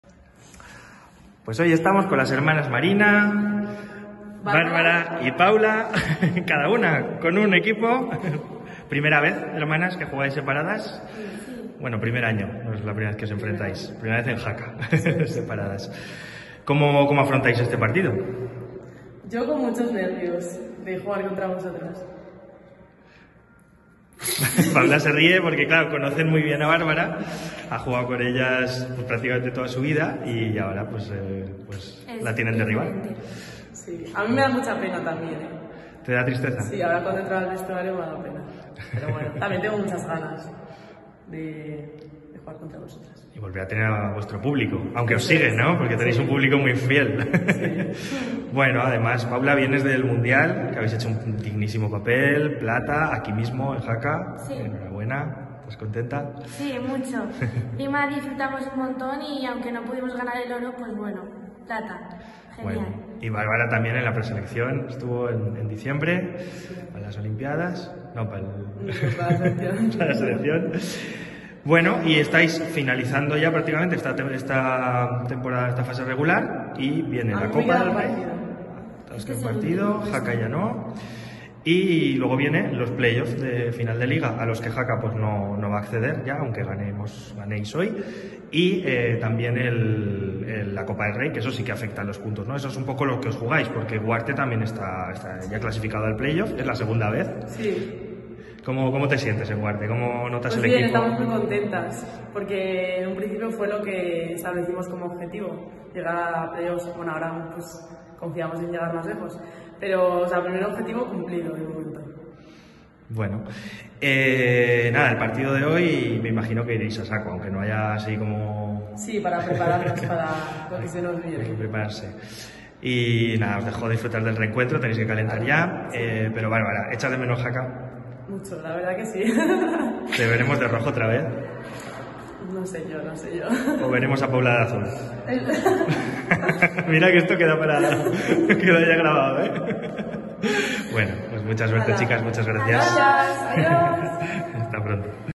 Entrevista a las hermanas Marina (Liga Iberdrola Hockey sobre Hielo)
Con motivo del encuentro de la Liga Ibedrola se reúnen las dos hermanas Marina que esta temporada juegan una en cada equipo. Paula, que es la máxima goleadora del Club Hielo Jaca y ganó la medalla de plata en el mundial U18 jugado en Jaca este mes y Bárbara que es partícipe de la segunda clasificación a Play Offs de la historia del Kosner Huarte y que participó en diciembre de la preselección con el equipo nacional.